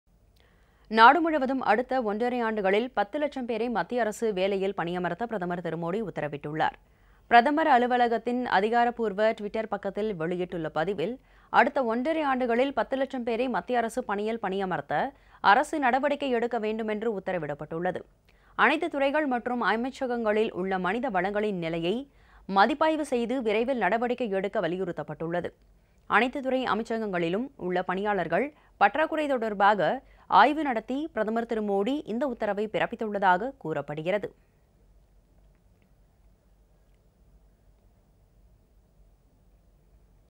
அடுத்த ஒன்றரை ஆண்டுகளில் 10 லட்சம் பேருக்‍கு மத்திய அரசு பணி | Modi | Central Government Job
அடுத்த ஒன்றரை ஆண்டுகளில் 10 லட்சம் பேருக்‍கு மத்திய அரசு பணி | Modi | Government School Admission